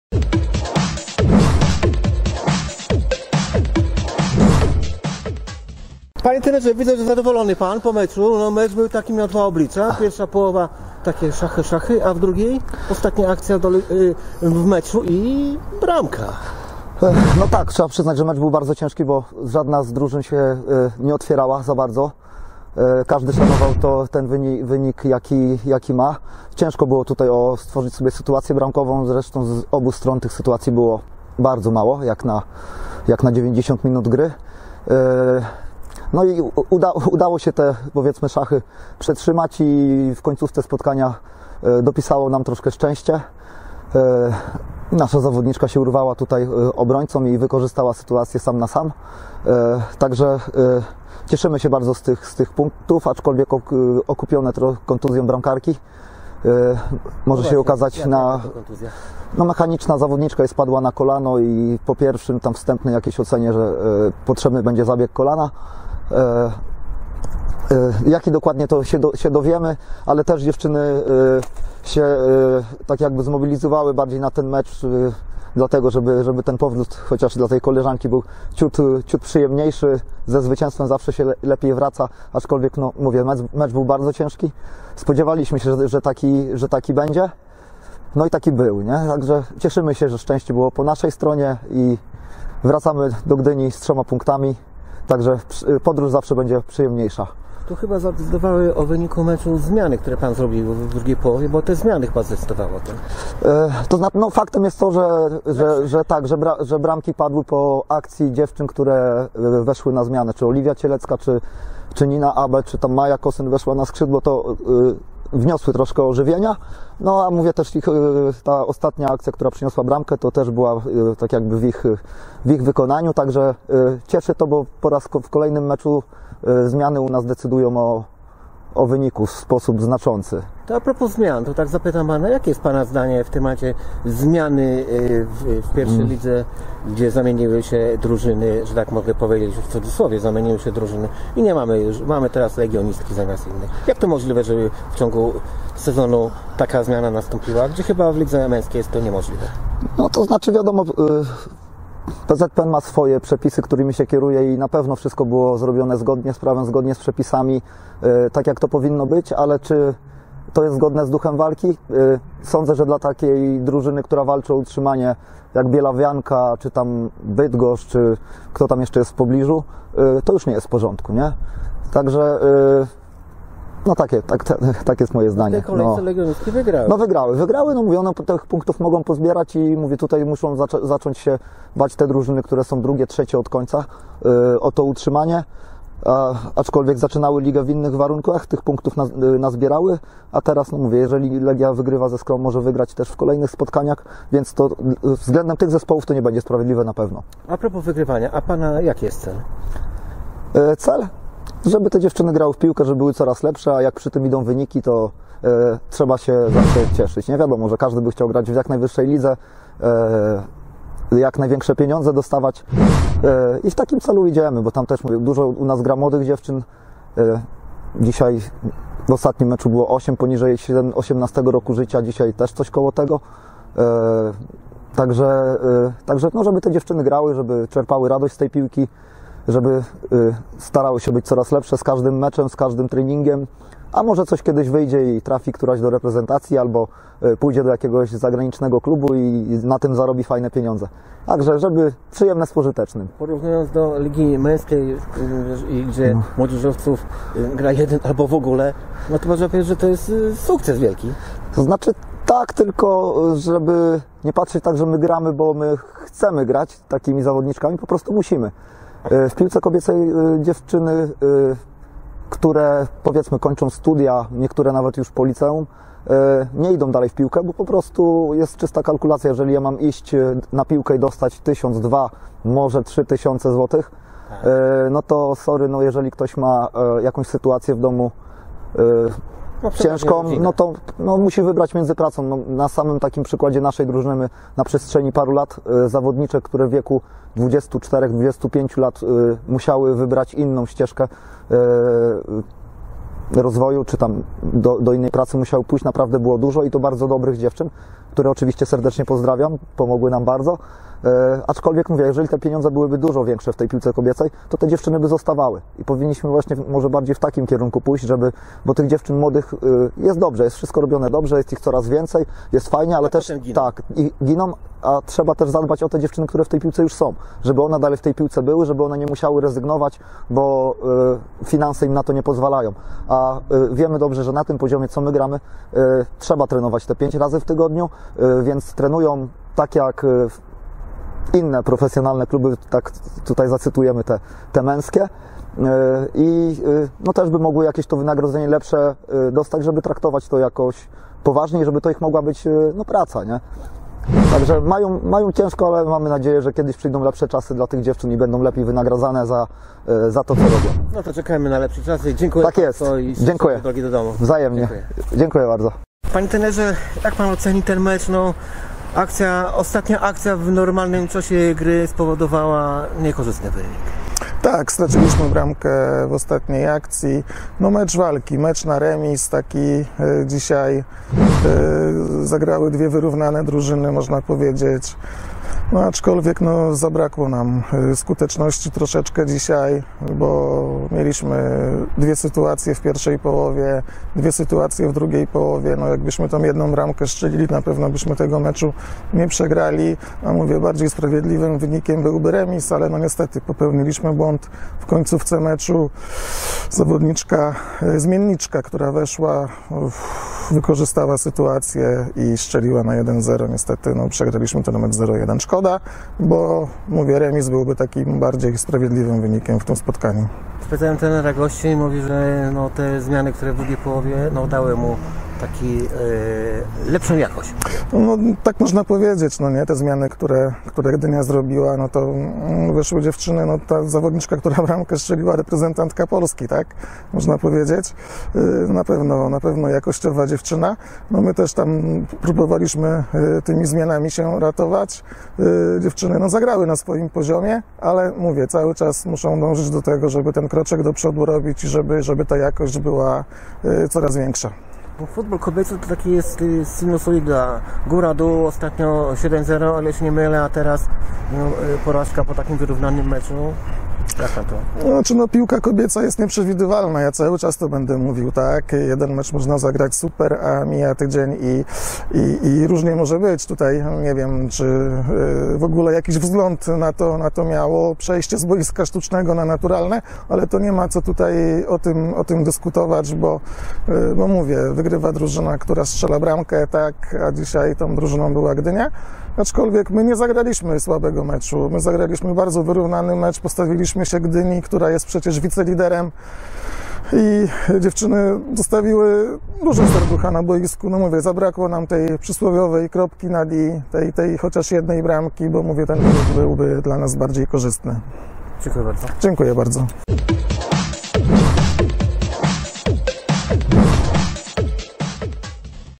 Wywiady po meczu HydroTruck Radom Checz Marcus Gdynia Robert Chałaszczyk i Wojciech Pawłowski
Wywiady po meczu HydroTruck Sportowa 4 Radom  -  Energa  Checz  Marcus Gdynia  Robert Chałaszczyk i Wojciech Pawłowski